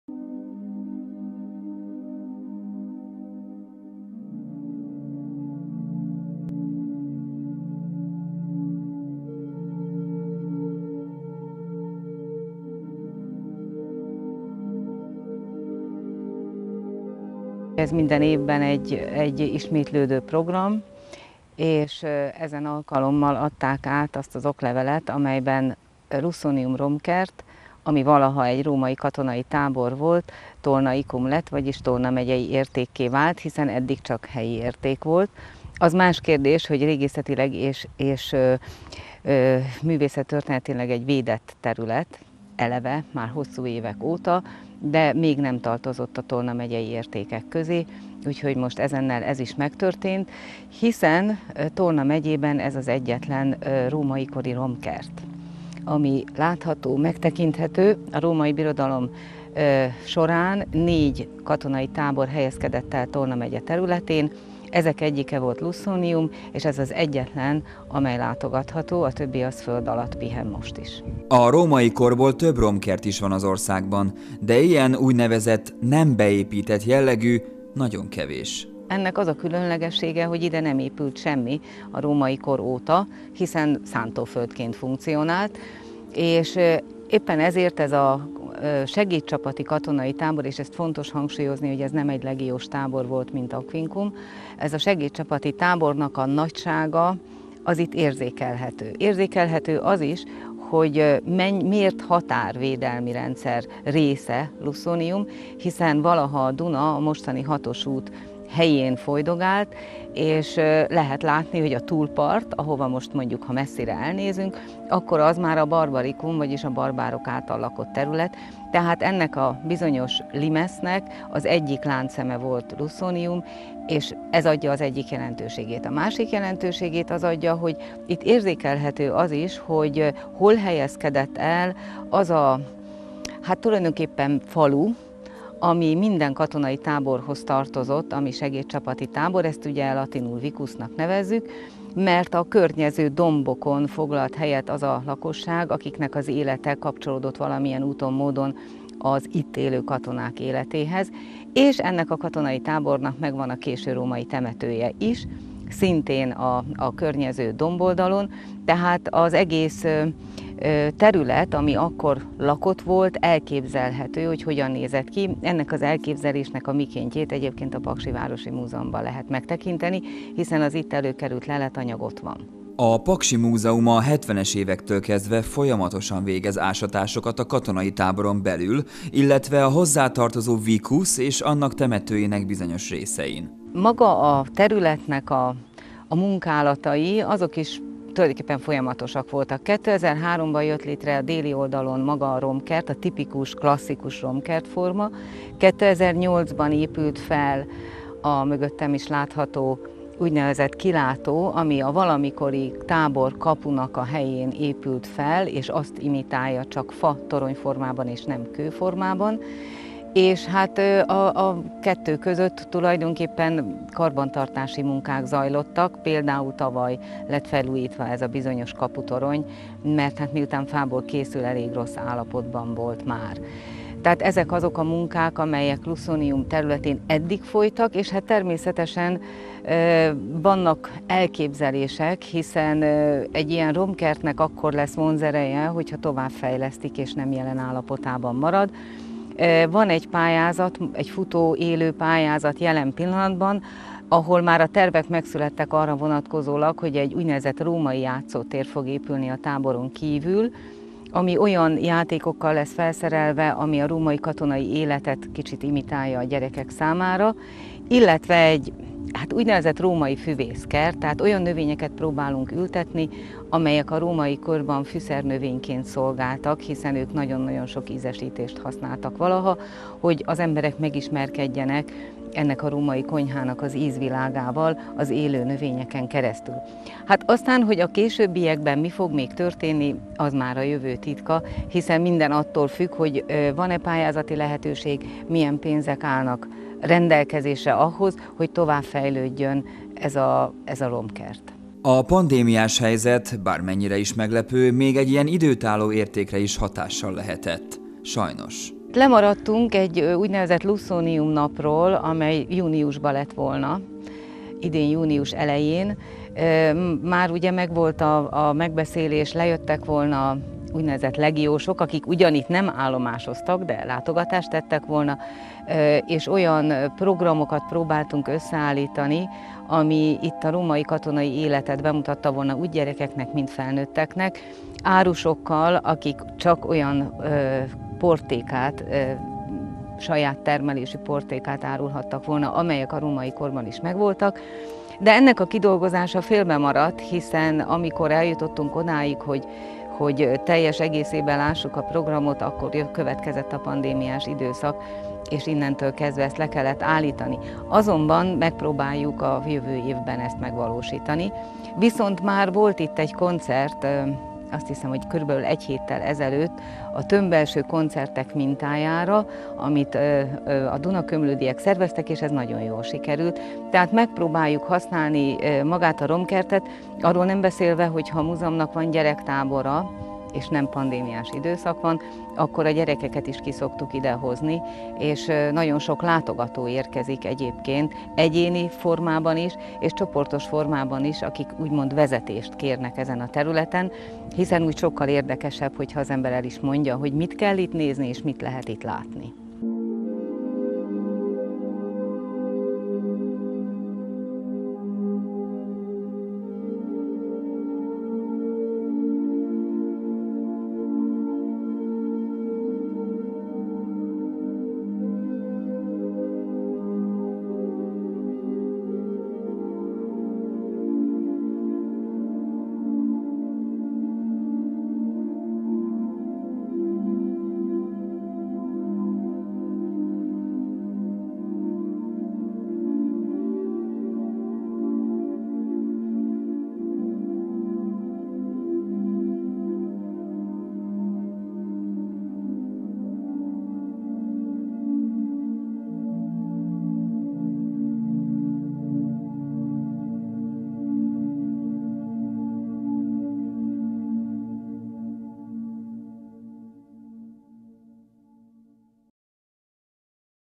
Lussonium Tolnaikum
A Tolna megyei Önkormányzat Vár a megye című rendezvényén osztja ki a megyei értékdíjakat. Július elsején, Medinán: Tolnaikum minősítést kapott az egykori katonai tábor, Lussonium is. A római kori romkert a Paksi Városi Múzeum irányítása alá tartozik.